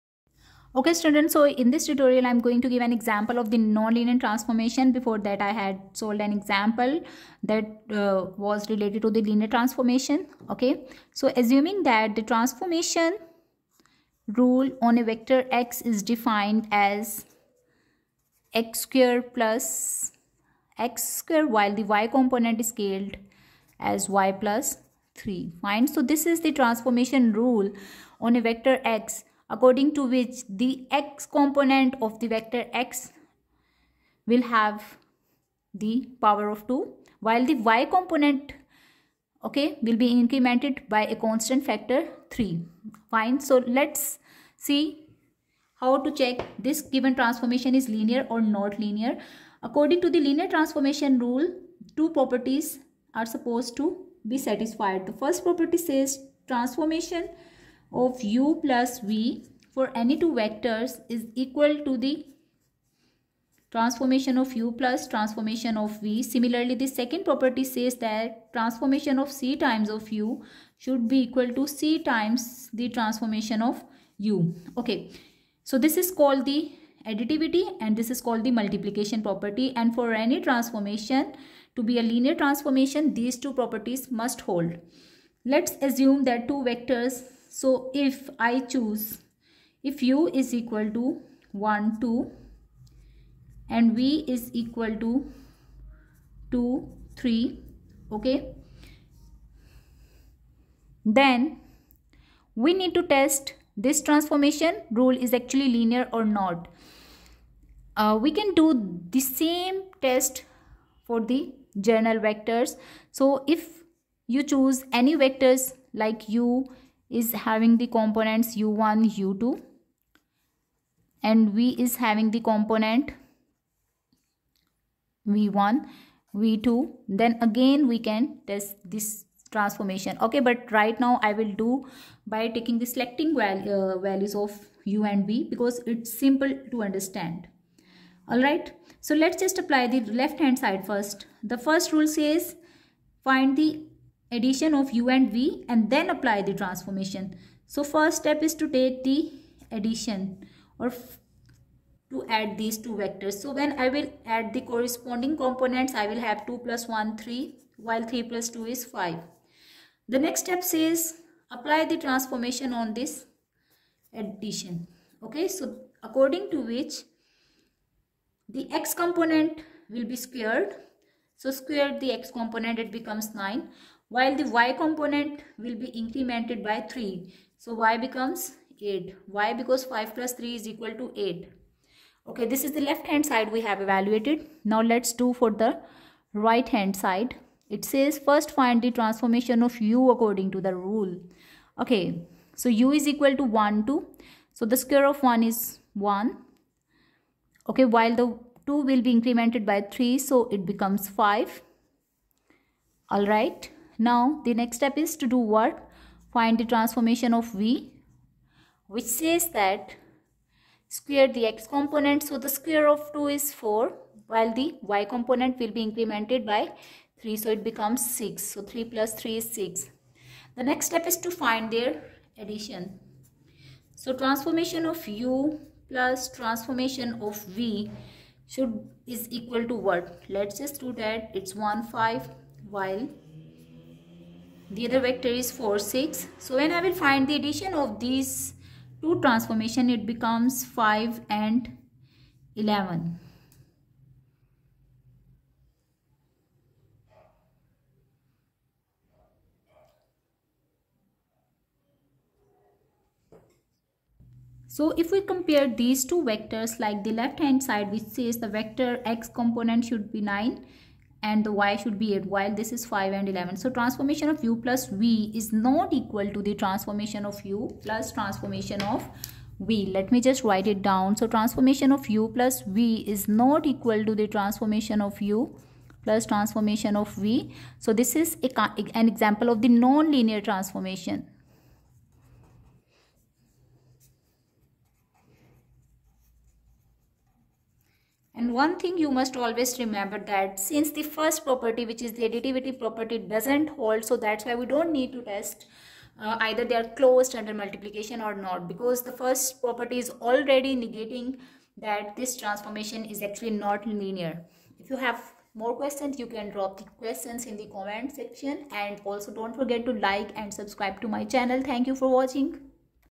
How to check the given Transformation is Non-Linear ? || Example of Non-Linear Transformation-Part 3
How to check the given Transformation is Non-Linear ?